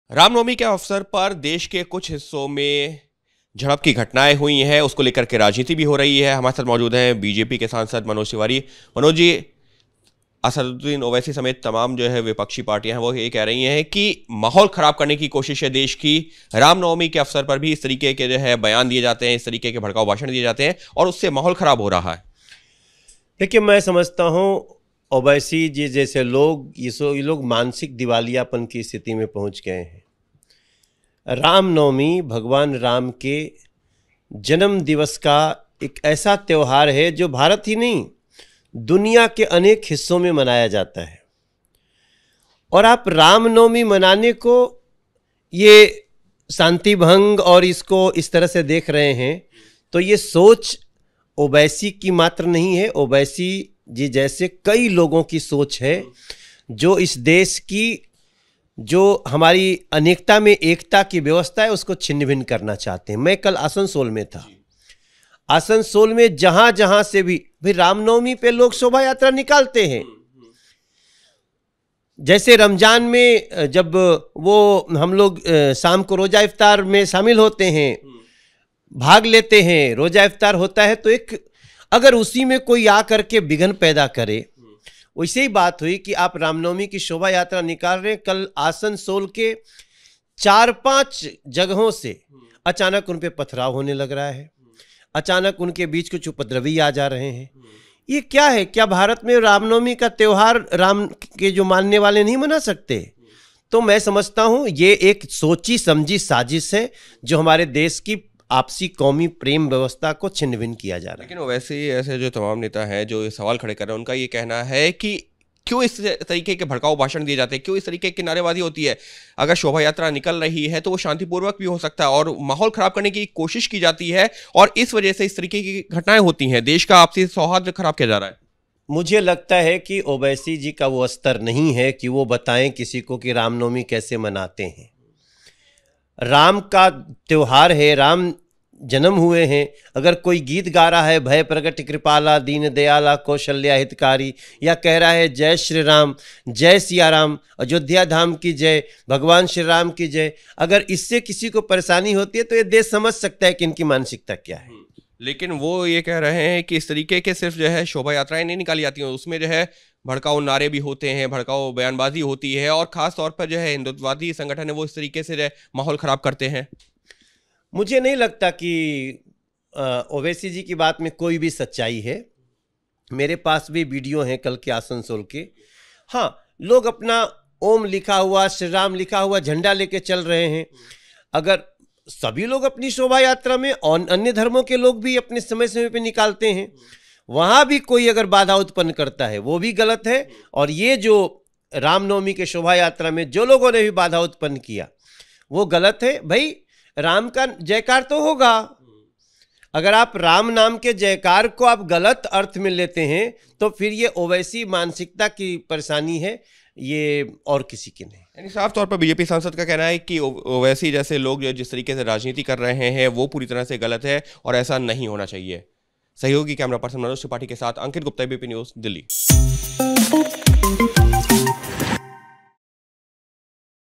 Owaisi पर Bjp नेता Manoj Tiwari ने साधा निशाना कहा, वो मानसिक दिवालियापन की हालात में पहुंच गए हैं
ओवैसी जैसे लोग मानसिक दिवालियापन की स्थिति में पहुंच गए हैं रामनवमी का त्यौहार दुनिया के अनेक हिस्सों में मनाया जाता है . ये सोच सिर्फ ओवैसी की नहीं हैं बल्कि ऐसे कई लोगों की है.क्या रामनवमीं का त्यौहार हिंदू लोग नहीं मना सकते हैं. ओवैसी का वो स्तर नहीं है कि वो बताएं कि रामनवमी को कोई कैसे मनाएं. अगर किसी को श्रीराम के नाम लेने से किसी को समस्या होती है तो देश समझ सकता है कि उनकी मानसिकता कैसी है.